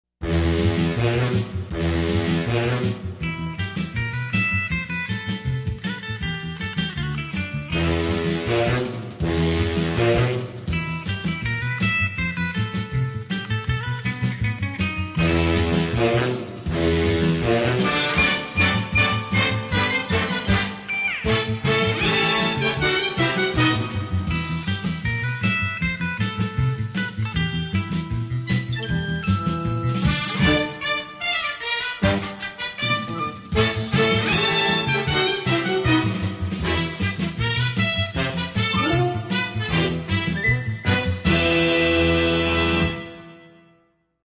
Avengers as Classic Movie Monsters as an 80's Sitcom Comedy
Thanks for taking the time to check out my AI generated version of Avengers as Classic Movie Monsters as an 80's Sitcom Comedy! These 80's style screen grabs are created using  images generated with MidJourney.

I'll still be making animations, but let me know if you'd like to see more of these videos.